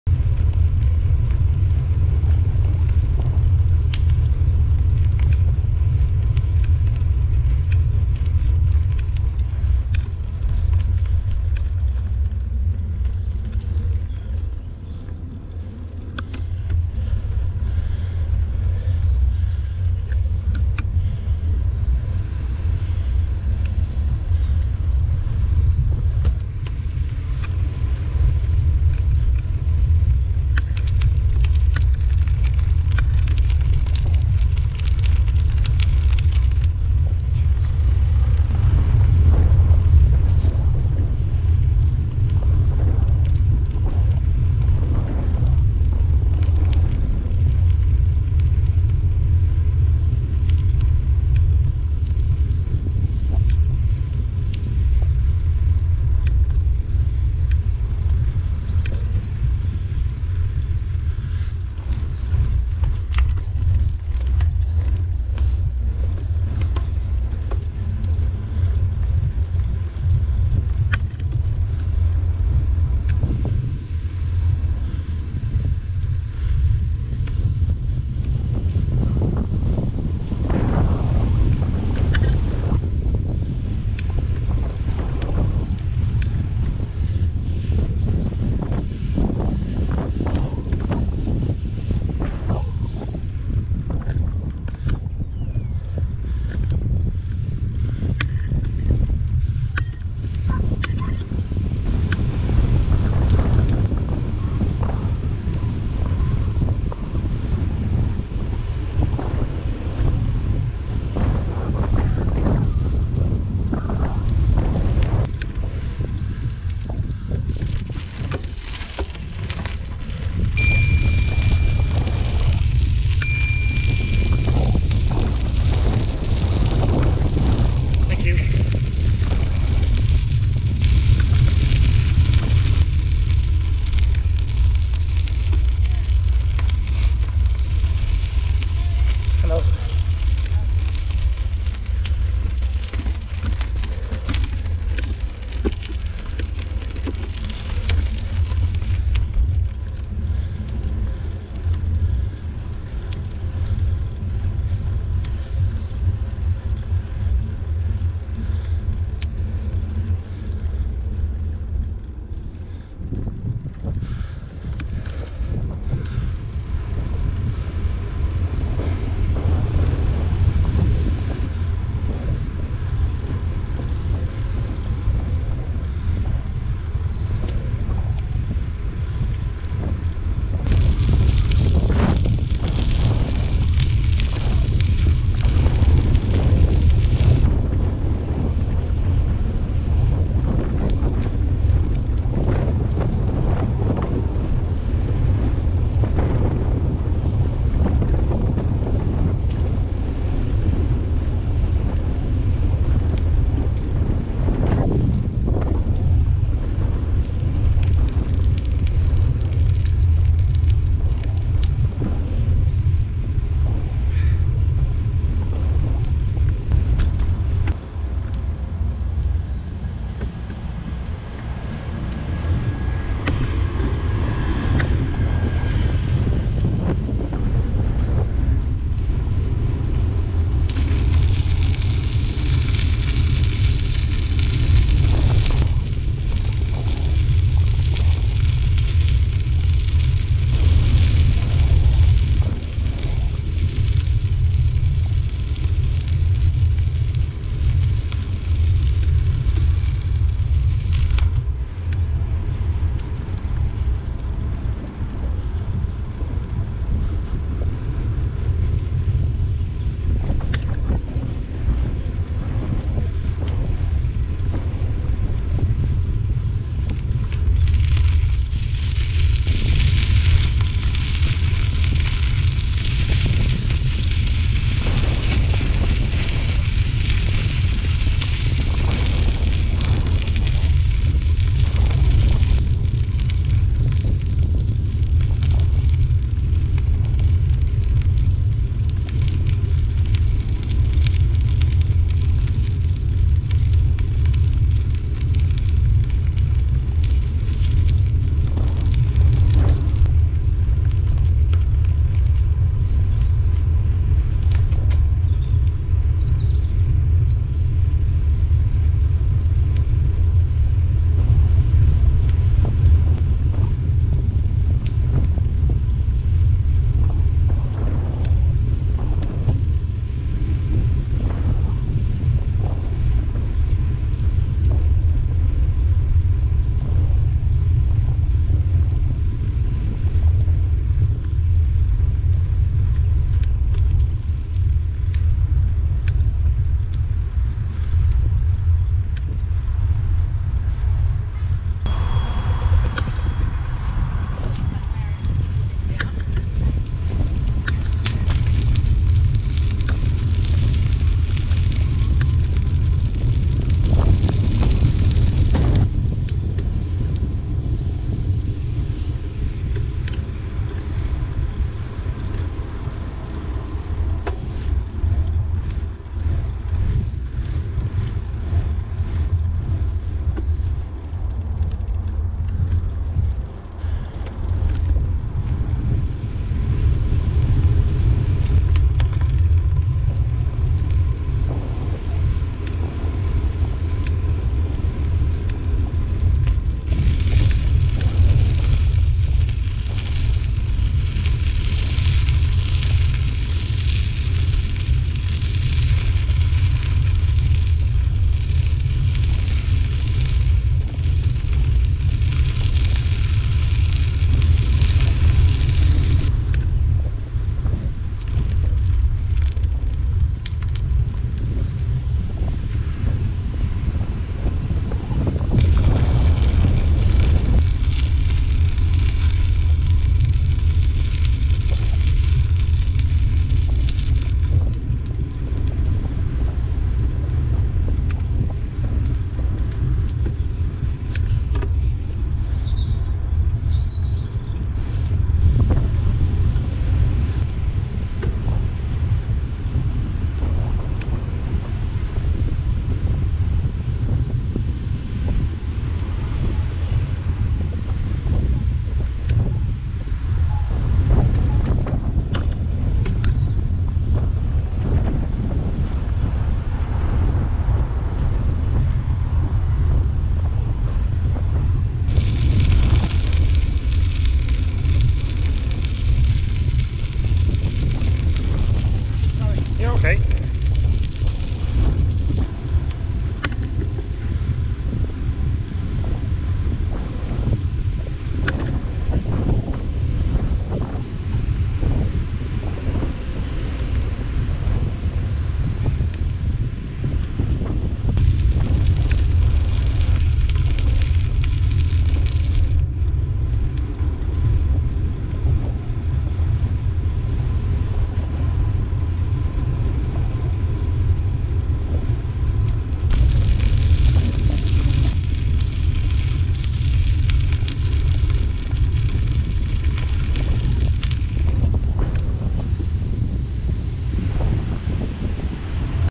Mullingar to Maynooth (Nanny Quinns)
Take 2 - without a puncture this time.

From Mullingar to Maynooth. Filming started at Nanny Quinns